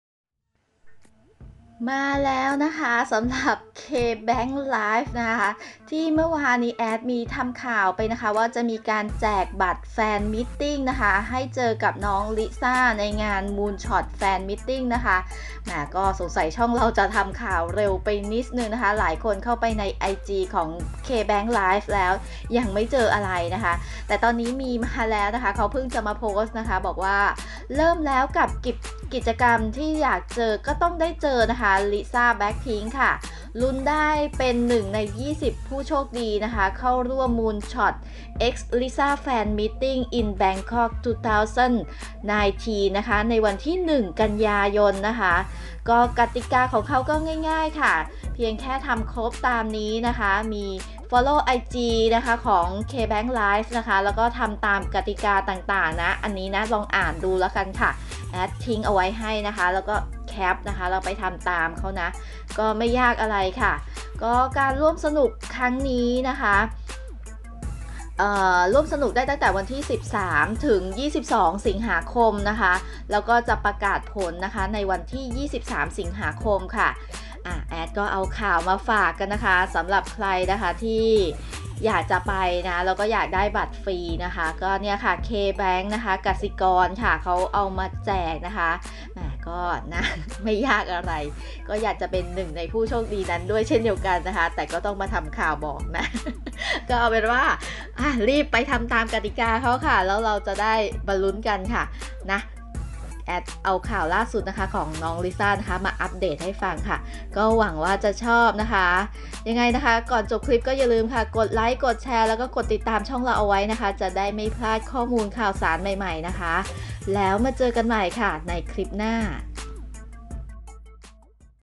ลิซ่า blackpink มาแล้ว กสิกรใจปล้ำแจกบัตรแฟนมีตติ้ง20ใบ
กสิกรจัดBig surprise แจกบัตรแฟนมิตติ้งพบ ลิซ่า blackpink ฟรี
ลิซ่าblackpinkpantip
ลิซ่า blackpink
ลิซ่า
ลลิสา มโนบาล
อัพเดทข่าวล่าสุด blackpink